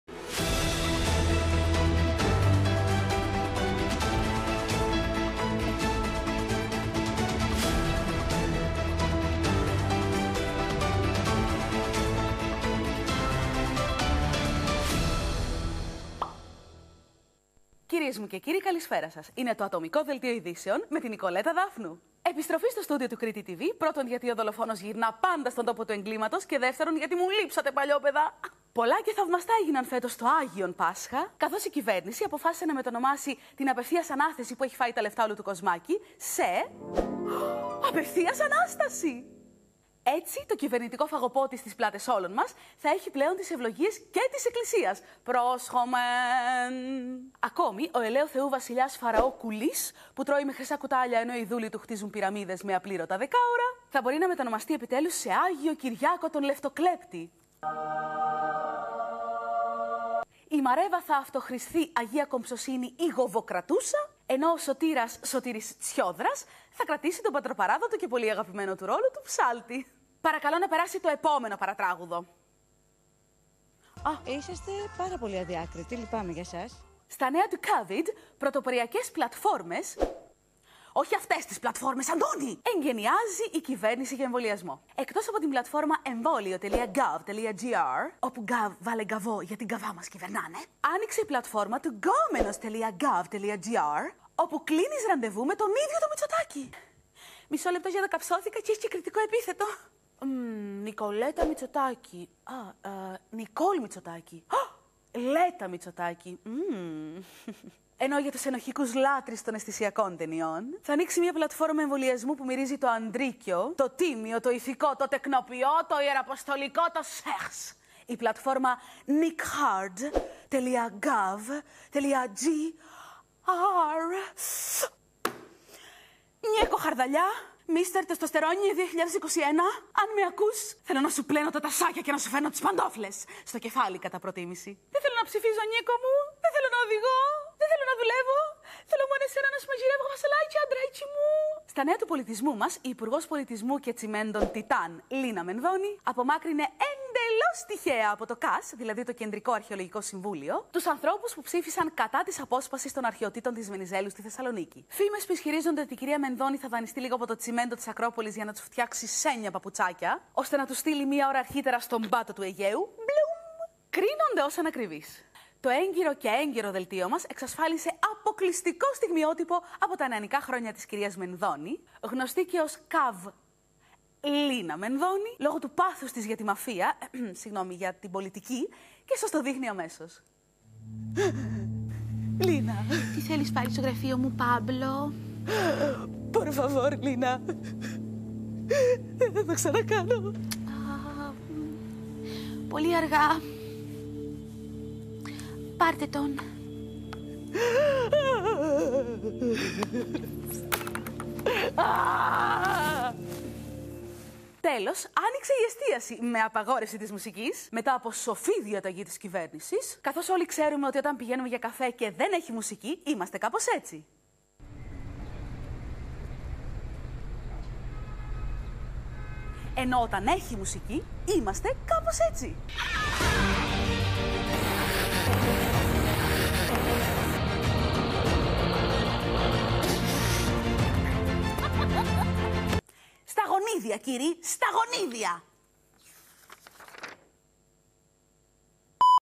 Ατομικό Δελτίο Ειδήσεων με την Νικολέτα Δάφνου #14
Η Νικολέτα Δάφνου σχολιάζει την επικαιρότητα με τον δικό της μοναδικό τρόπο!